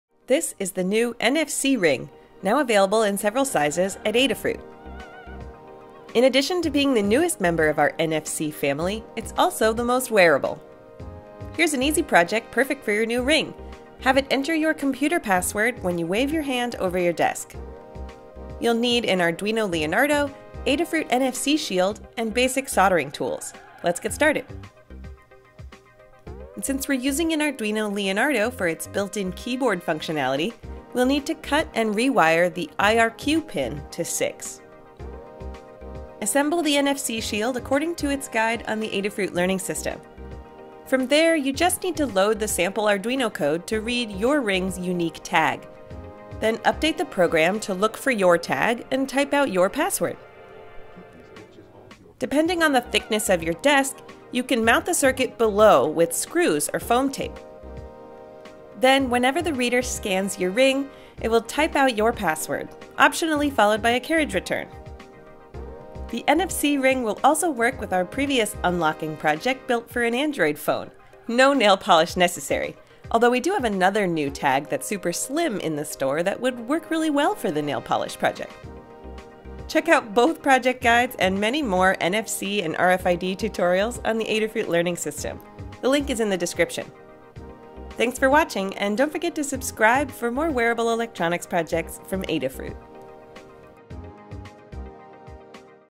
NFC Ring Password Helper
Log in to your computer with a swipe of your hand! Use an NFC ring and reader to type in your password for you with an Arduino Leonardo, which can act like an HID keyboard.